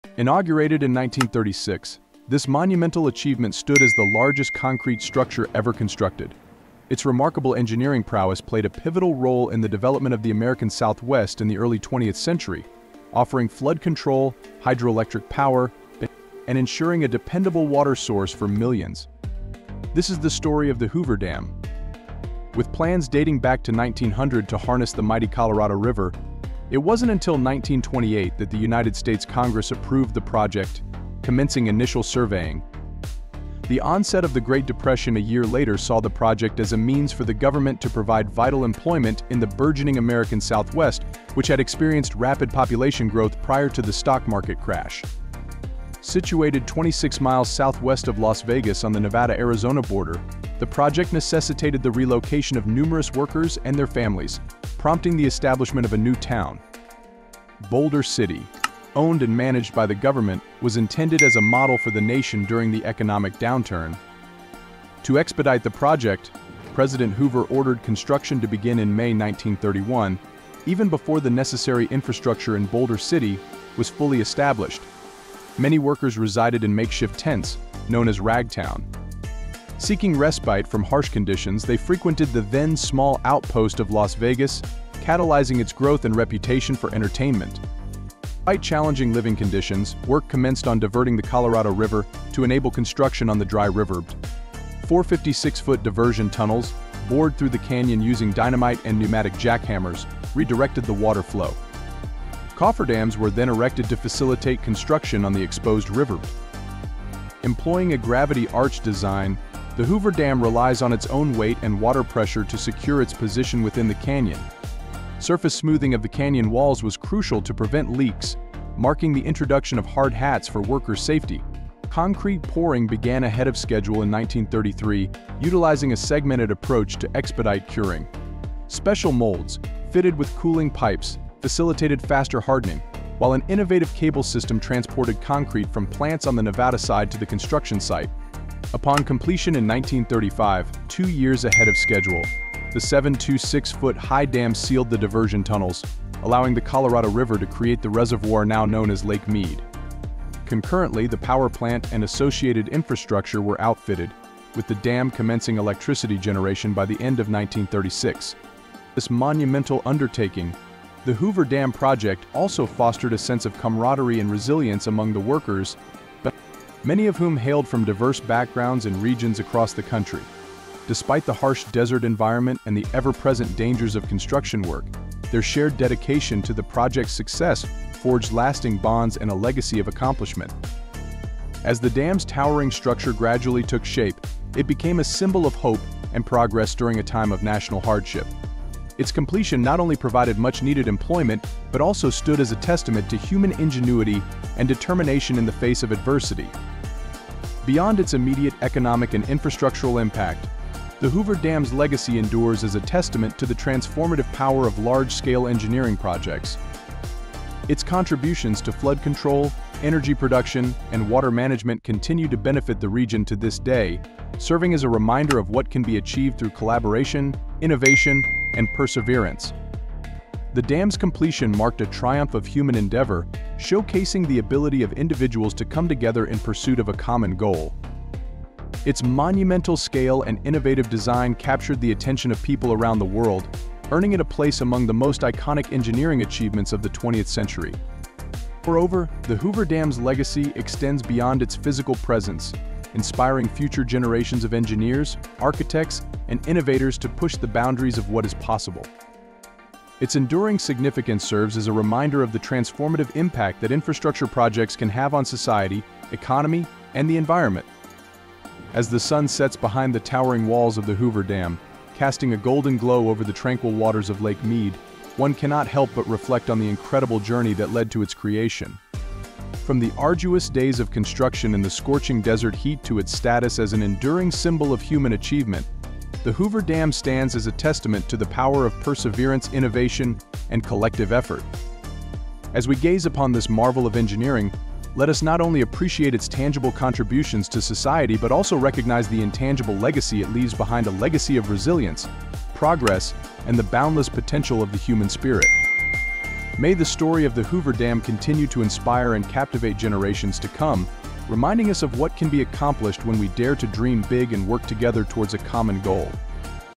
HOOVER DAM | Building The Hoover Dam
"Embarking on the monumental task of reconstructing the Spitallamm dam in Switzerland, engineers navigate intricate challenges to safeguard national infrastructure. Witness the integration of cutting-edge construction techniques and environmental consciousness in the creation of Lake Grimsel's hydroelectric power plant, a vital source of renewable energy. Explore the complexities of dam construction, disaster prevention, and the influence of geological stability on major infrastructure endeavors, mirroring the ambitious spirit of the Hoover Dam's construction."